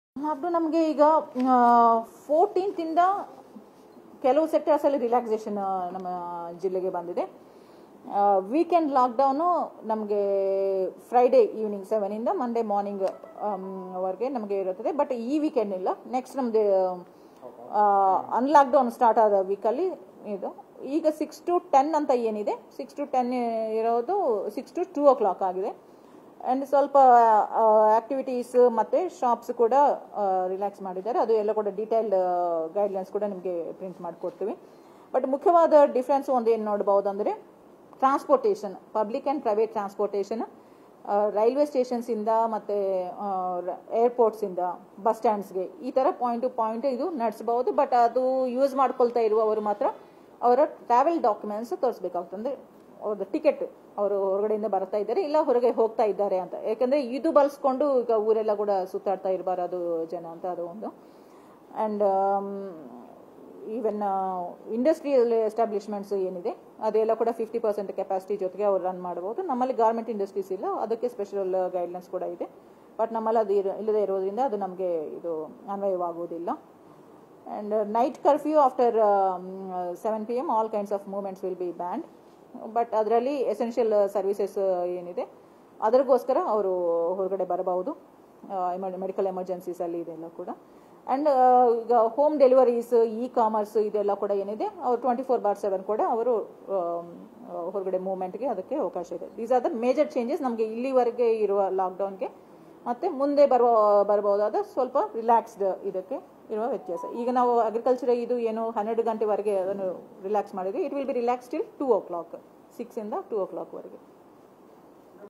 ಜೂನ್ 14 ರಿಂದ ಕಲಬುರಗಿ ಜಿಲ್ಲೆಯಲ್ಲಿ ಅನ್ ಲಾಕ್ ಕುರಿತು ಕಲಬುರಗಿಯ ಜಿಲ್ಲಾಧಿಕಾರಿ ವಿ.ವಿ.ಜೋತ್ಸ್ನಾ ಮಾಹಿತಿ
ಜೂನ್ 14 ರಿಂದ ಕಲಬುರಗಿ ಜಿಲ್ಲೆಯಲ್ಲಿ ಹಂತ ಹಂತವಾಗಿ ಅನ್ ಲಾಕ್ ಪ್ರಕ್ರಿಯೆ ಪ್ರಾರಂಭವಾಗುವುದರ ಕುರಿತು ಕಲಬುರಗಿಯ ಜಿಲ್ಲಾಧಿಕಾರಿ ವಿ.ವಿ.ಜೋತ್ಸ್ನಾ ಮಾಹಿತಿ